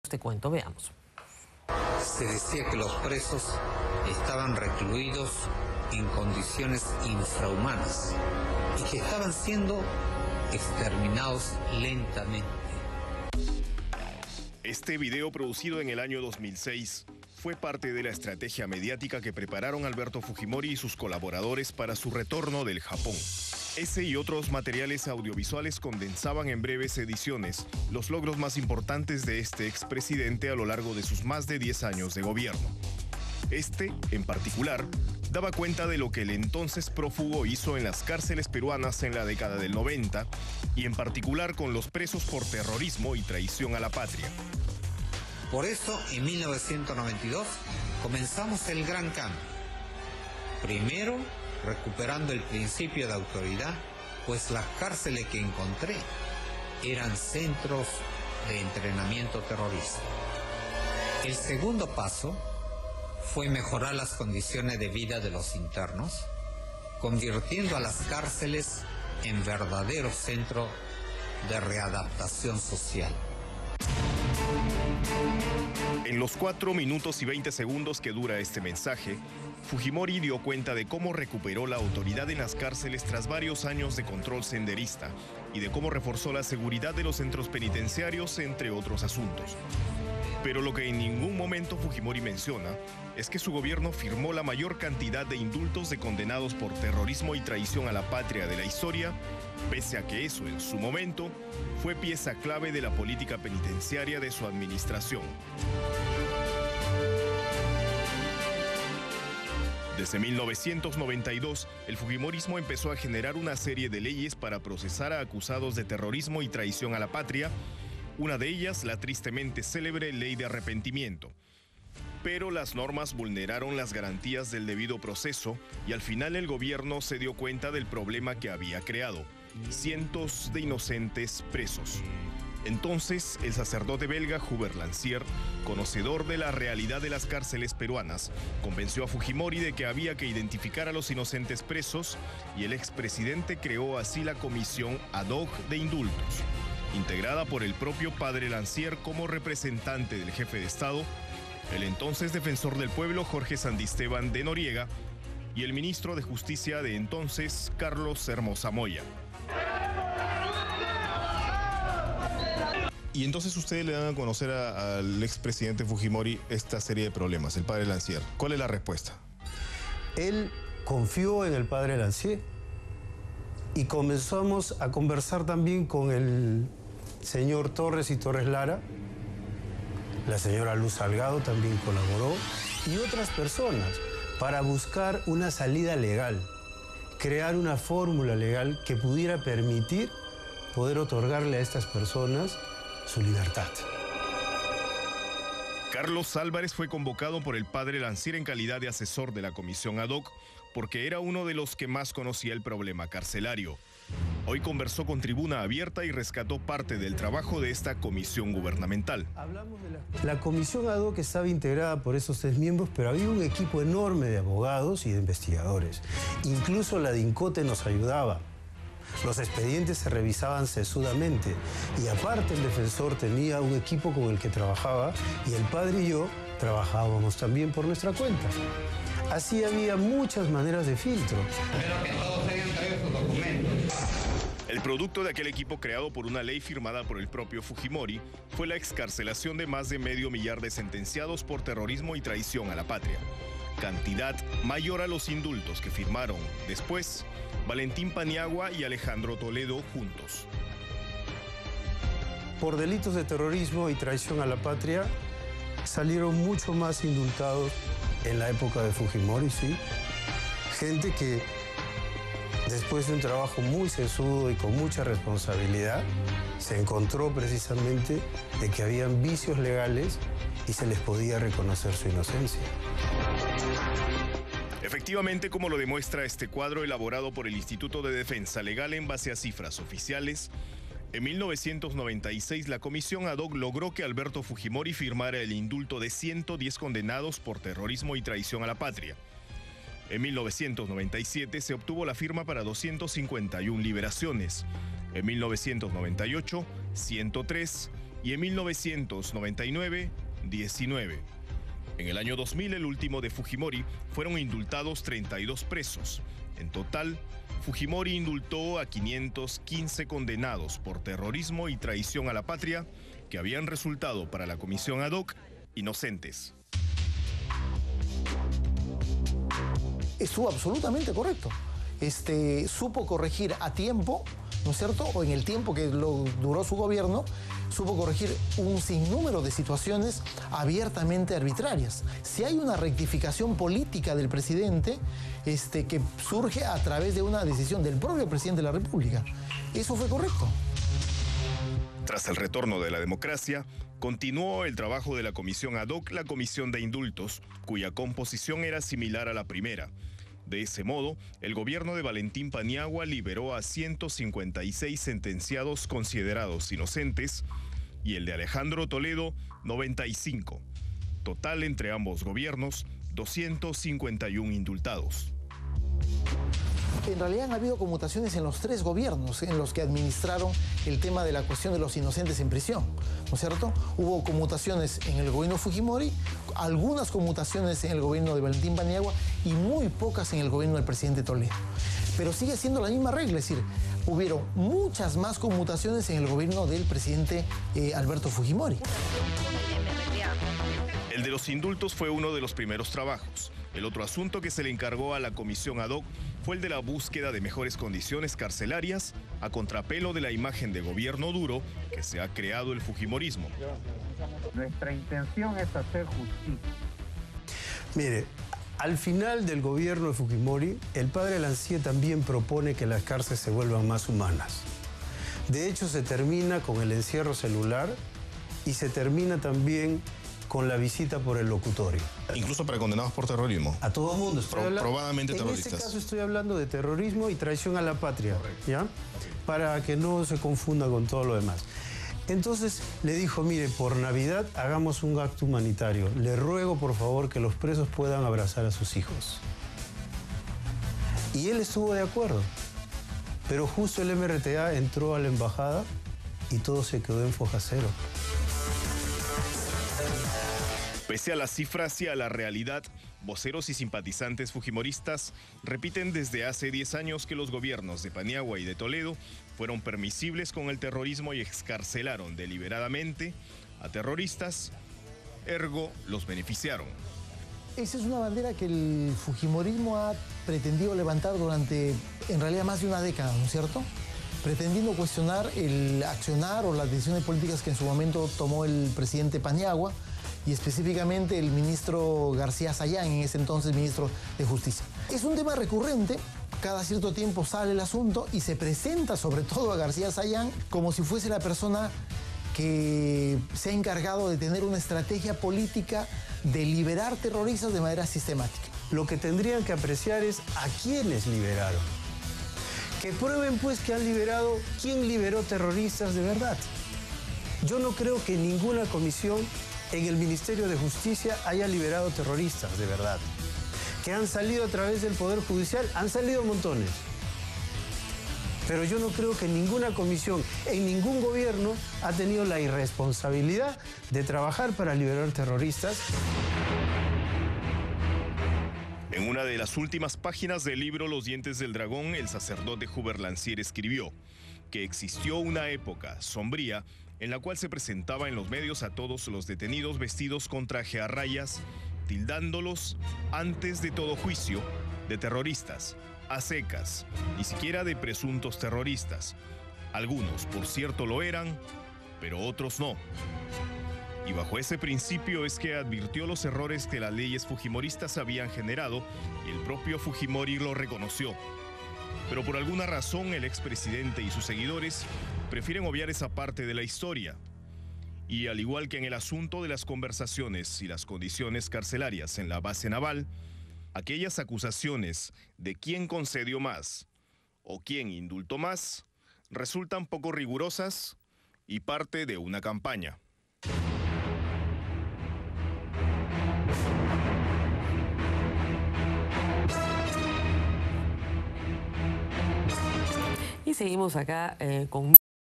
Hablan las cifras: Gobierno Fujimorista liberó a más acusados por terrorismo
Fuente: Global tv